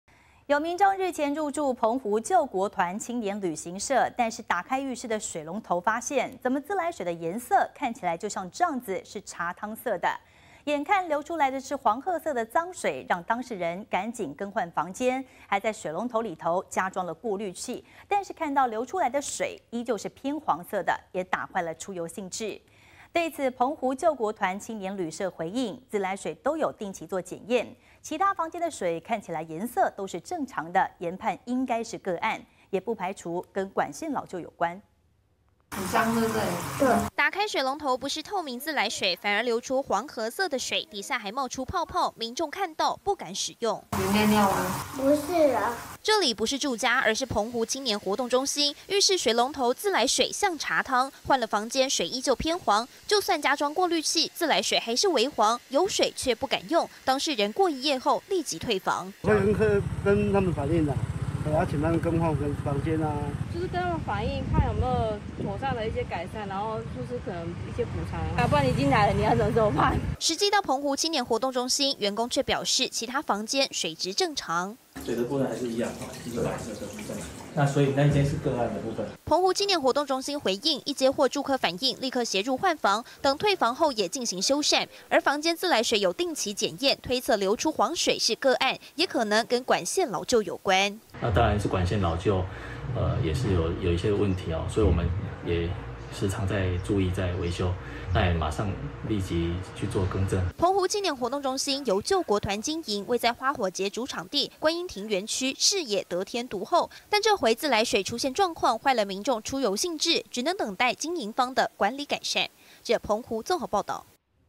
加裝過濾器水質仍偏黃 緊急退房壞了遊興
有民眾日前入住，澎湖救國團青年旅社，結果打開浴室水龍頭，自來水顏色像茶湯，眼看流出來的是黃褐色的髒水，當事人更換房間，還在水龍頭加裝過濾器，流出來的水，依舊偏黃，隔日辦理住宿，也壞了出遊興致。對此，澎湖救國團青年旅社回應，自來水有定期檢驗，其他房間水的顏色正常，研判是個案，也不排除跟管線老舊有關。